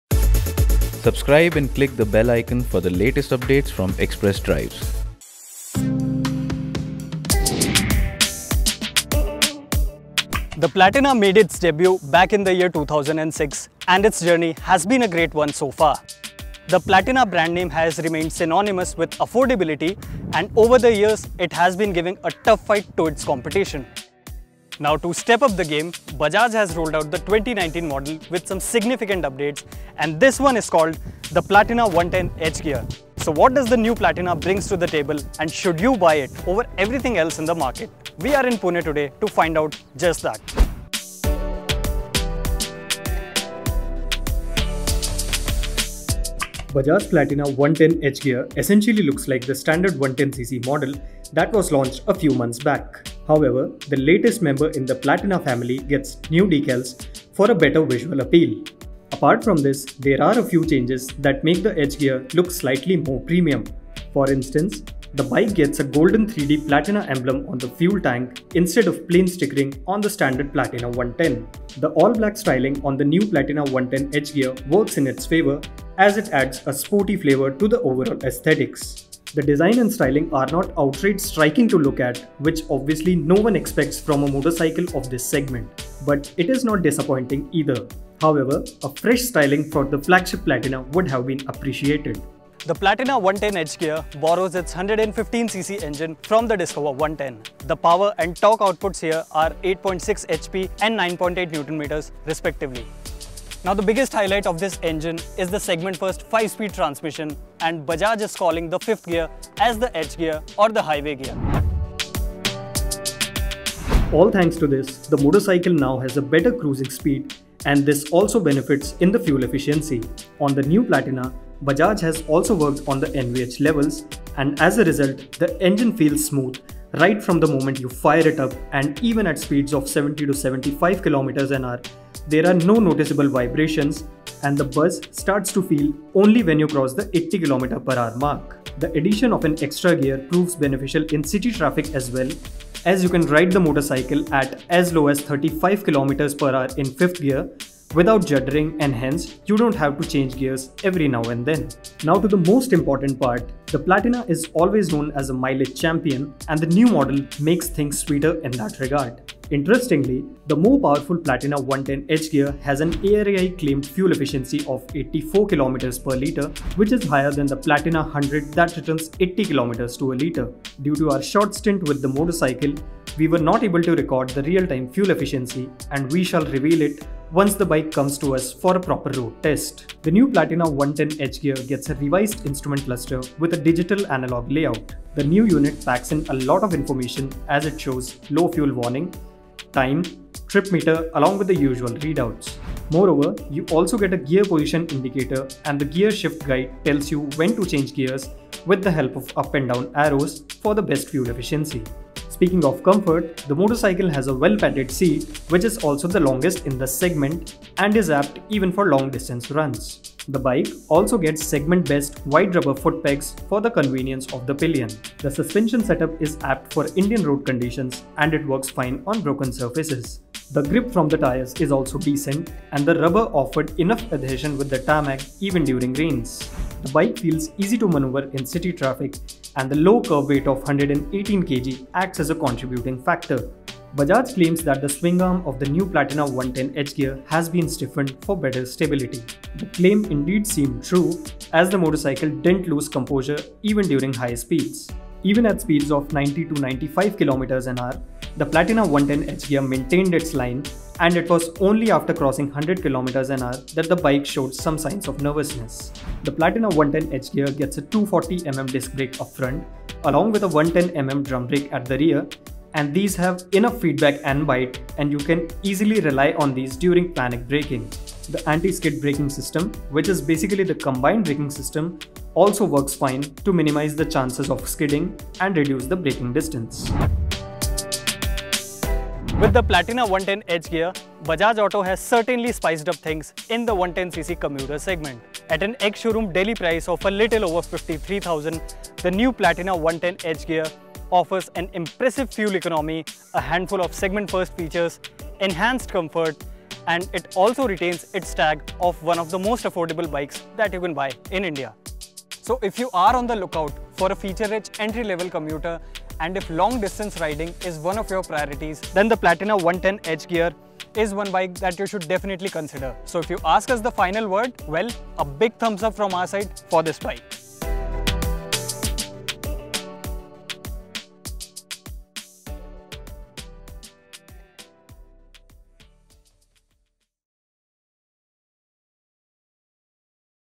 Bajaj Platina 110 H-Gear Review | Splendor Rival With Five Gears
Bajaj Platina 110 H-Gear, a 110cc motorcycle with an extra gear has been launched in the country recently and we were in Pune last week to ride it. The new Platina 110 H-Gear model is not just about a five-speed gearbox as there are multiple other updates that make the riding experience richer than before. So, what the Platina 110 H-Gear is all about and how is it to ride. Pradeep Shah tells you that in this review.